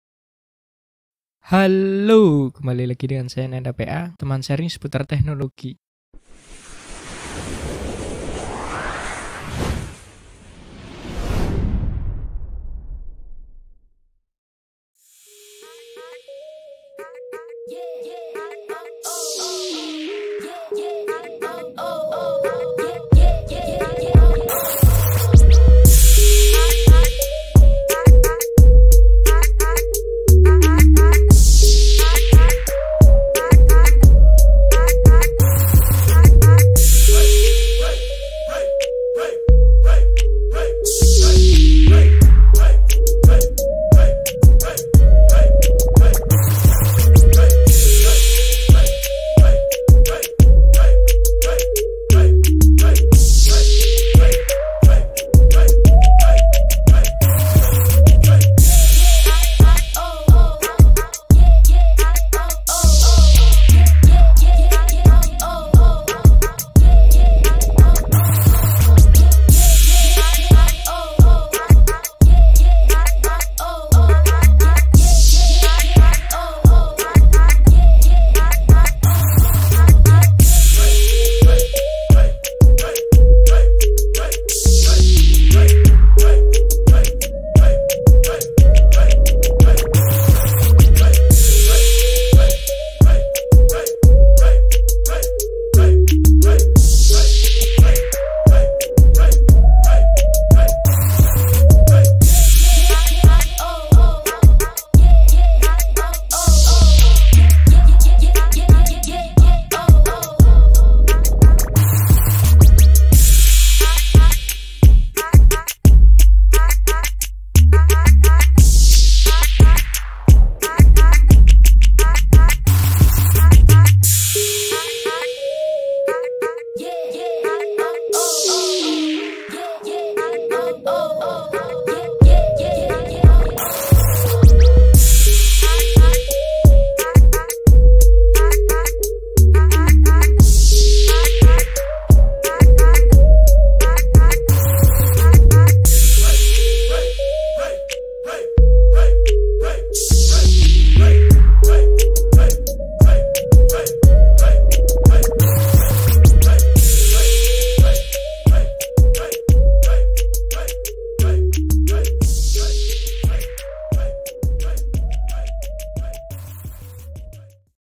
√ 7+ Rekomendasi Mouse Gaming 200 Ribuan Terbaik 2021 dengan Kualitas Gokil
Kamu seorang gaming ? Simak video 7 rekomendasi mouse gaming 200 ribuan 2021 dengan kualitas yang gokil.

Agar lebih MESRA dengan Saya